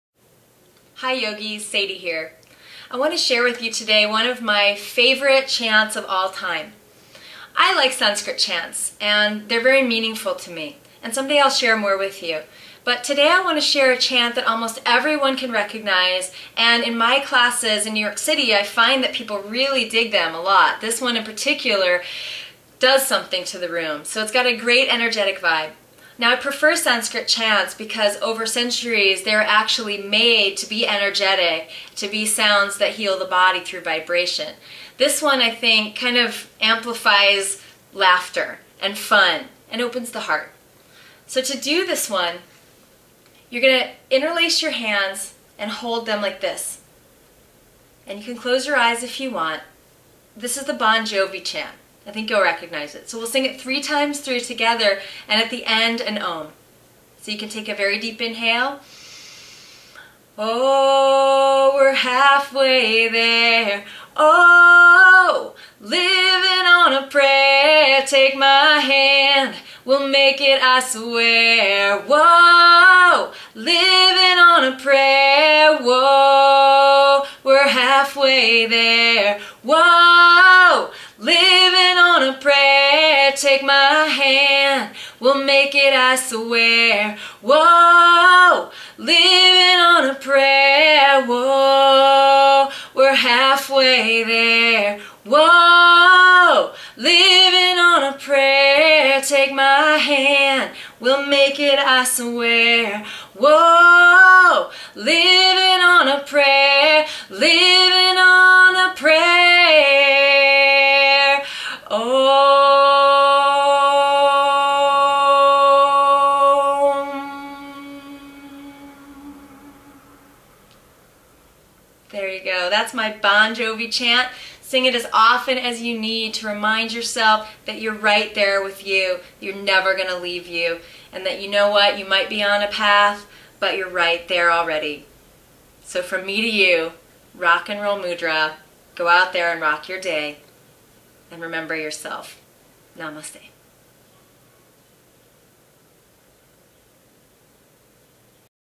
Yoga Stretch - THE BON JOVI CHANT
Yoga Stretch - MAGIC IS FOUND ANYWHERE, AND ALIGNING YOUR HEART AND MIND IS ONLY A BON JOVI SONG AWAY!  With master yoga trainer Sadie Nardini, Director of East West Yoga in NYC, and author of The Road Trip Guide to the Soul.